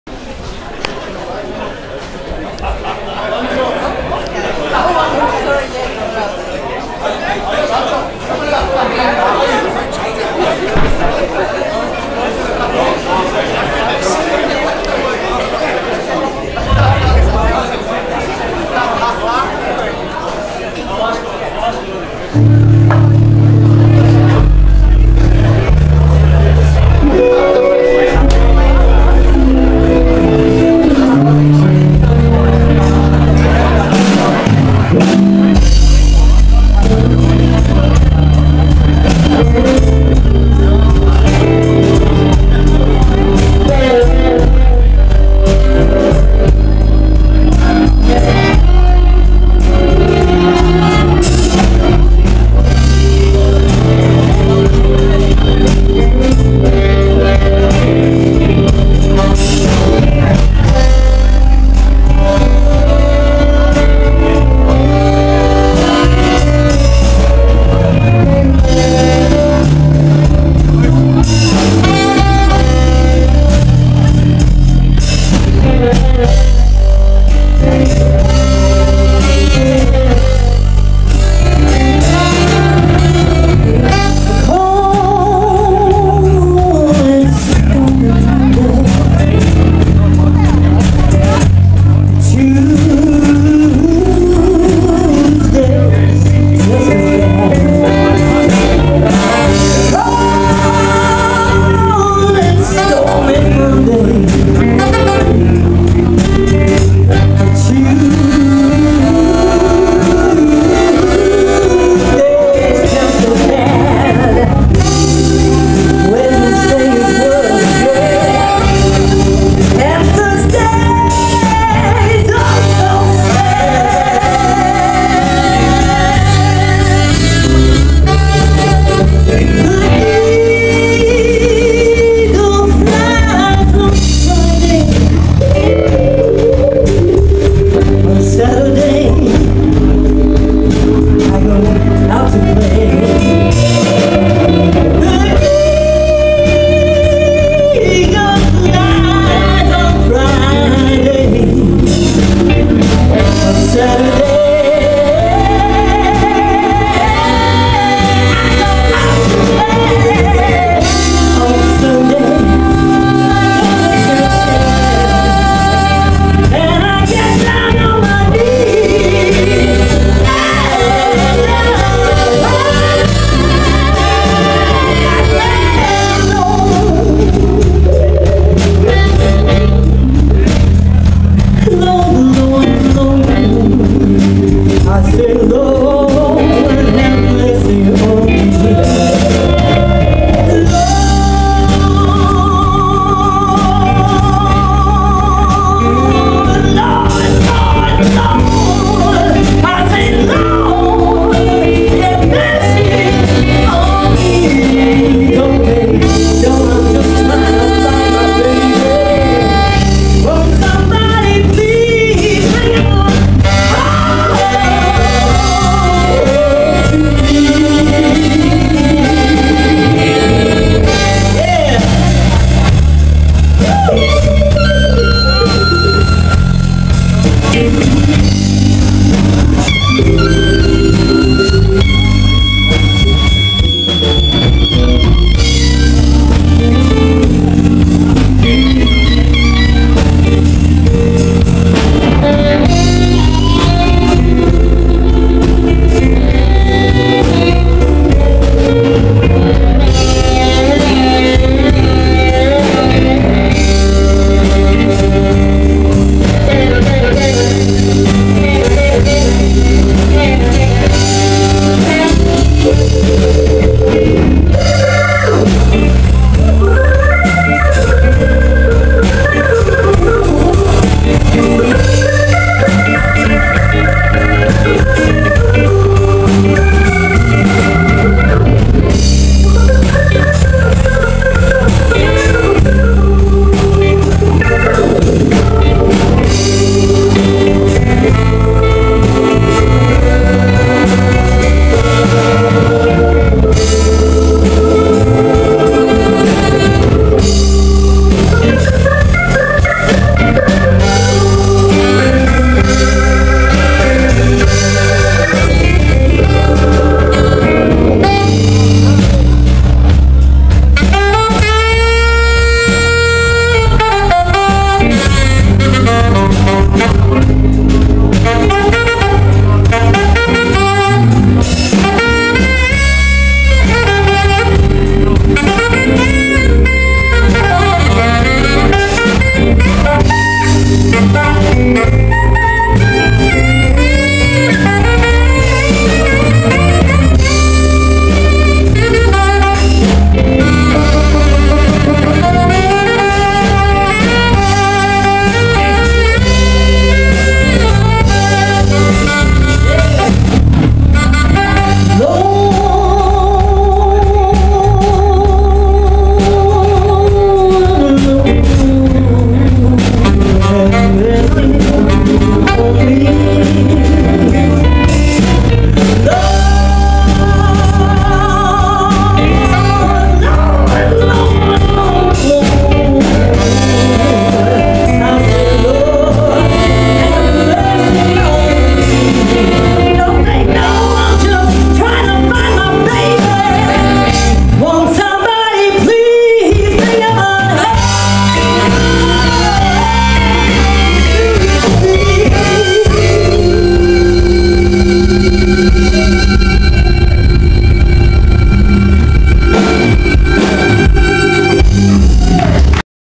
Marie McCormick Blues Band - Live @ The Ferry - Joined By FelixTheSaxCat
MMBB are playing @ The Ferry as part of the big blues festival in Glasgow, called Blues On River. FelixTheSaxCat has been joining the band for this occasion too.